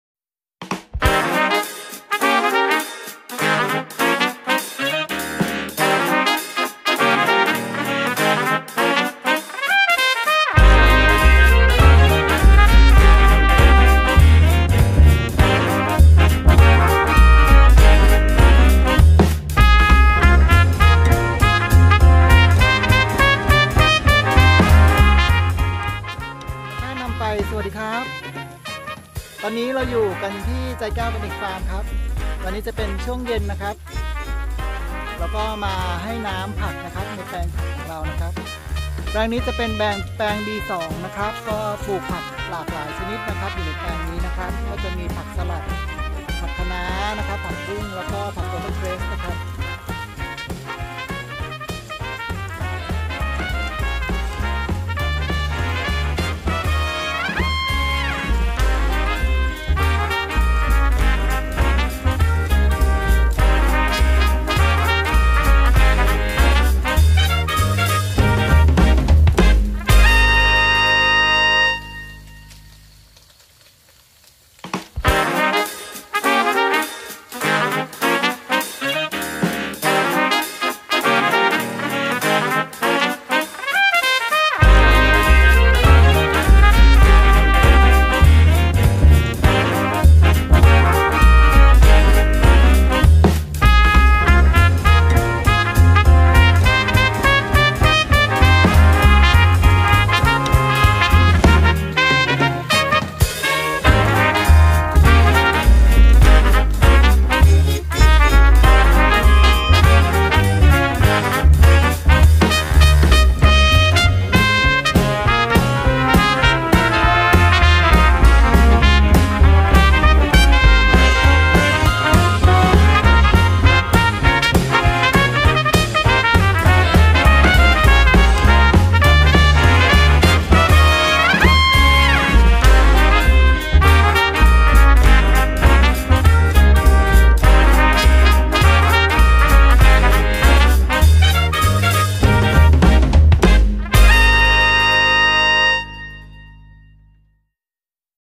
รดน้ำผัก@ธรรมชาตินำไปorganic
ผักไร้สาร ผักปลอดภัย ผักออร์แกนิค สวัสดีครับวันนี้ชวนเพื่อนๆมารดน้ำผักด้วยกันครับ @ใจแก้วออร์แกนิคฟาร์ม JAIKAEW ORGANIC FARM